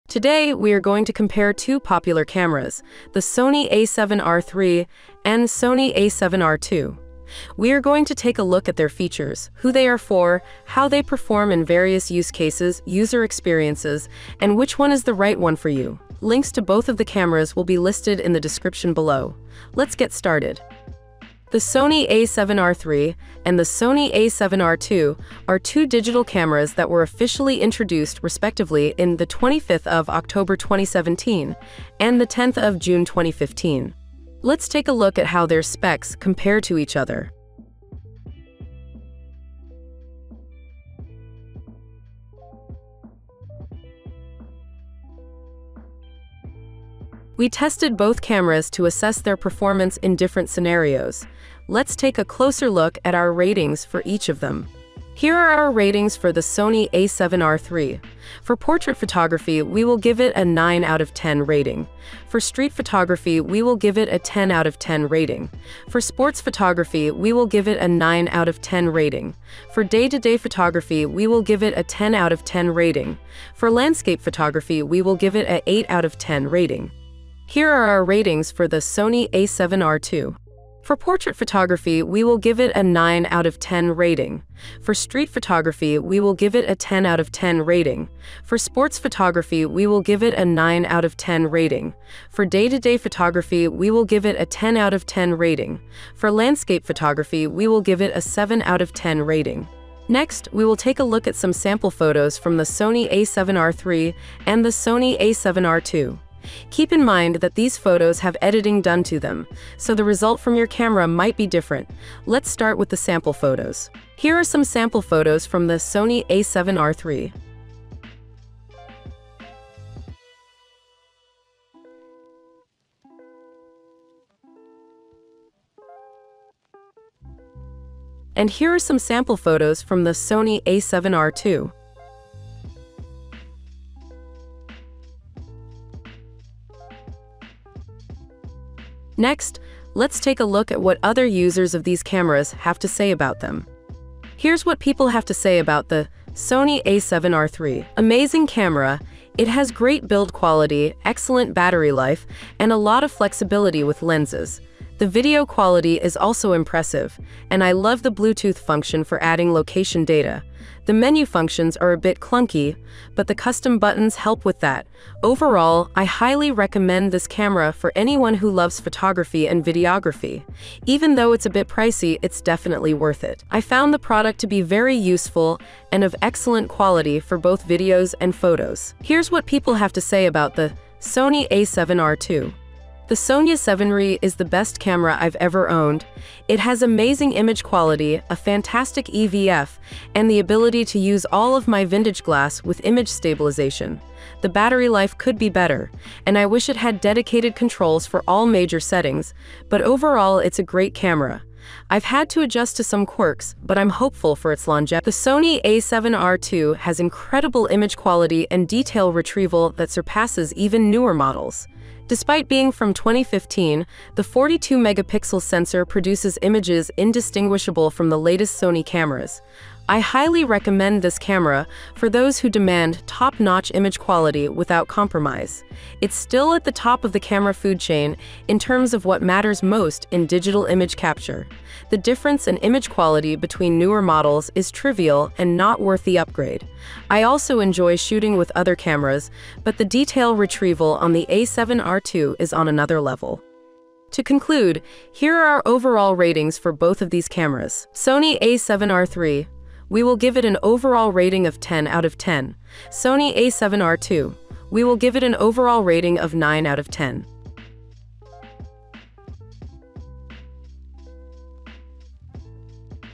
Sony A7R III vs Sony A7R II: Which Camera Is Better? (With Ratings & Sample Footage)
► Links to the latest prices of Sony A7R III and Sony A7R II:
► Sony A7R III
► Sony A7R II

 In this video, we did a head-to-head comparison of two popular cameras, the Sony A7R III and the Sony A7R II. We checked how they perform in street photography, portrait photography, landscape photography, day to day photography and sports photography. We also curated user reviews that already used the cameras from forums and compiled them.